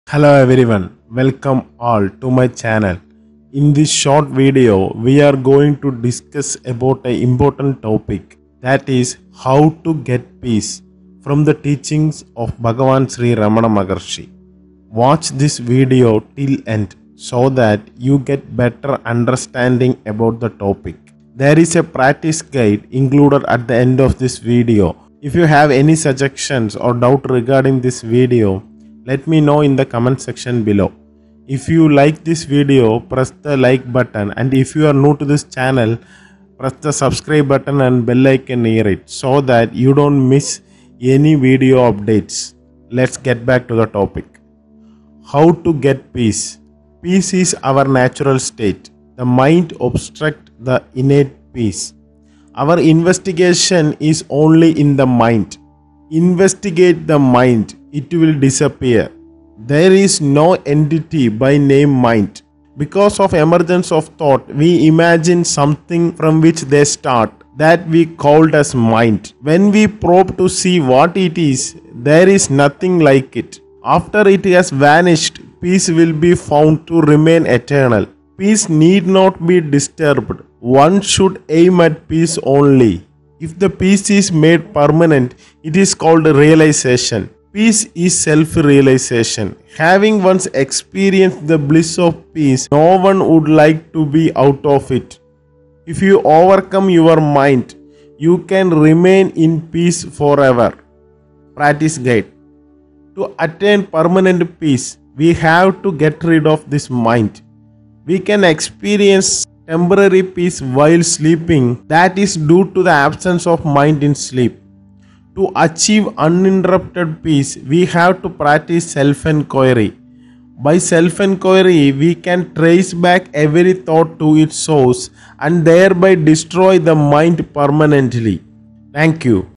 How to get Peace?, from the teachings of Bhagavan Sri Ramana Maharshi. A practice guide is included
In this video, the following topics are discussed.
1. What is peace ?
2. How to get peace?
3. What is mind?
4. How to get rid of the mind?
from the teachings of Bhagavan Sri Ramana Maharshi.